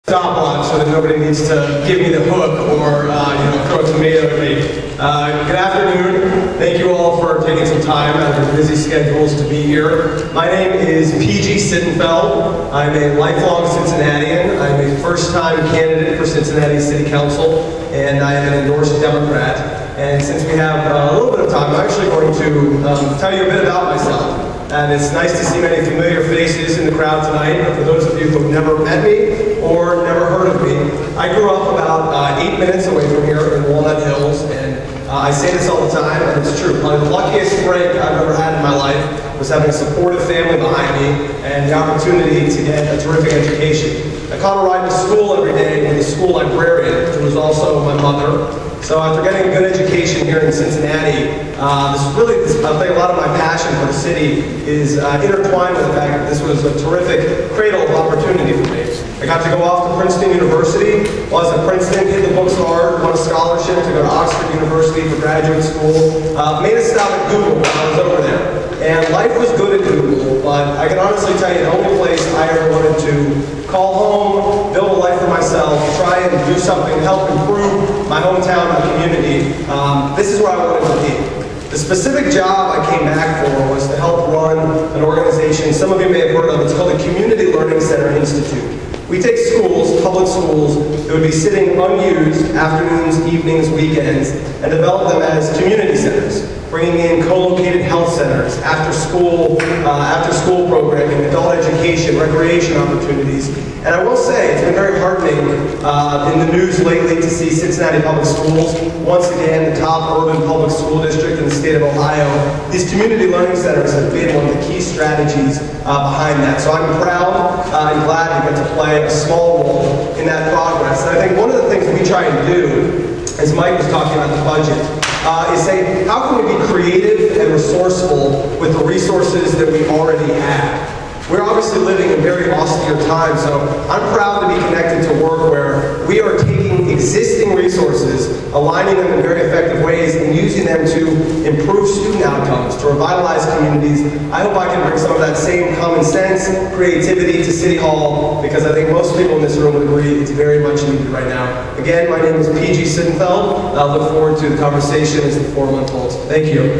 P. G . Sittenfeld, Candidate for Cincinnati City Council
At the  Community Issues Forum of September 15, 2011 the non-incumbent candidates for Cincinnati City Council will present  their case as to  why they should be elected.